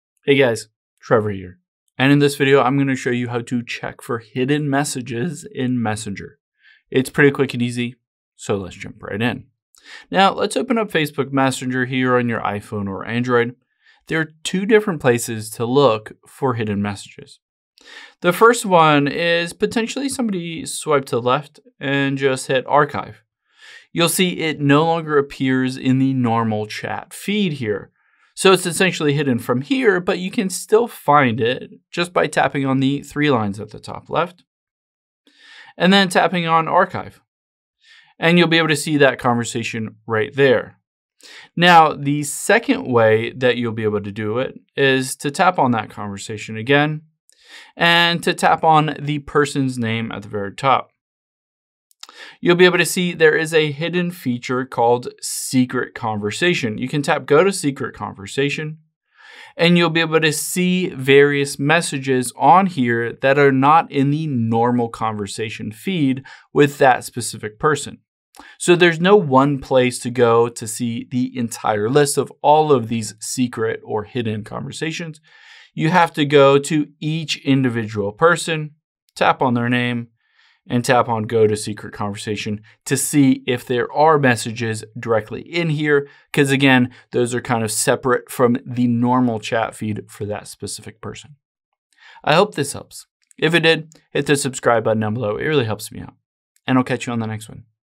How To Check For Hidden Messages In Messenger
Here's how to check your Facebook Messenger account for hidden messages and read them if you want.